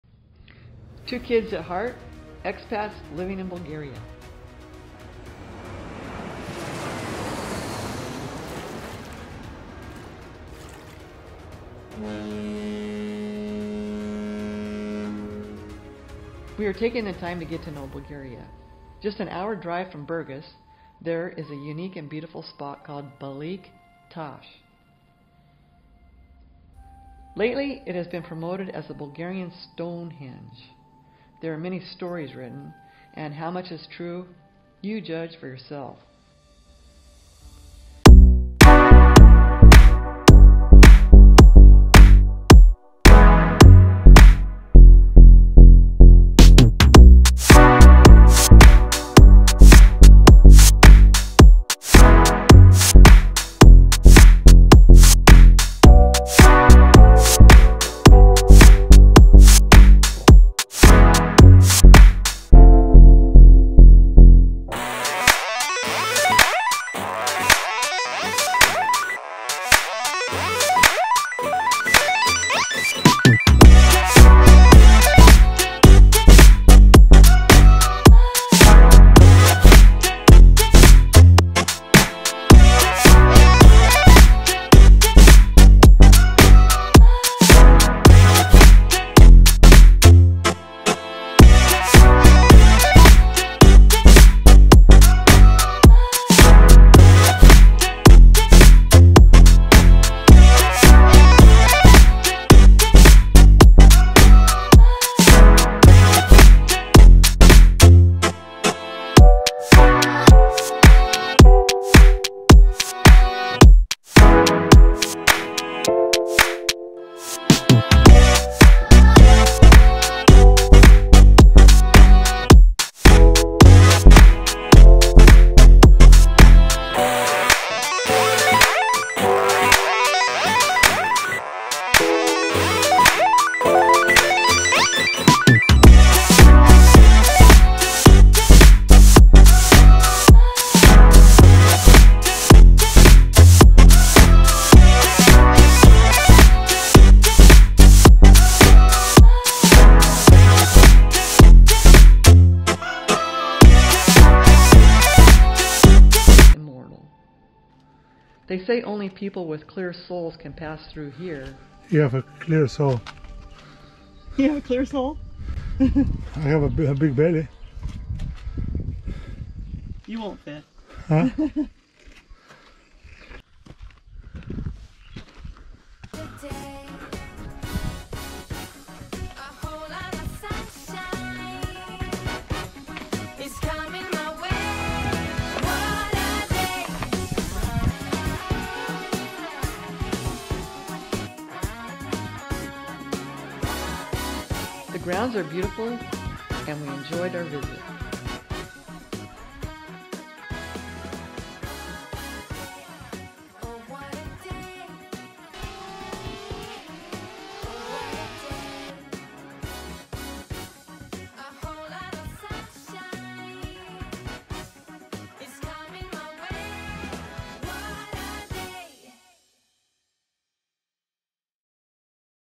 Bulgarian Stonehenge Baglik Tash
This video is about Baglik Tash, a Tharacian historic site located a short one hour drive from Burgas, Bulgaria just outside Primorsko.. There are many Legends about this 1300 years before Christ location. It is said that the Thracian's used this site for Cult like ceremonies and even, some how, were able to move 100 ton large Stones and piled them up on top of each other. There is a Thracian sun clock and a very narrow passage one can slide through and if they are skinny enough, they will fit and then have a "clear sole."

It is said to be the Stonehenge of Bulgaria. One needs to visit and see for themselves if they believe the legends. This place is historic however, our personal opinion is that the rocks are a natural formation over many many years. The setting is beautiful among an Oak forest and a large manicured field whereby families can visit, use the provided picnic tables for lunch and play in the large field area. It is maintained by the Burgas Historical Society.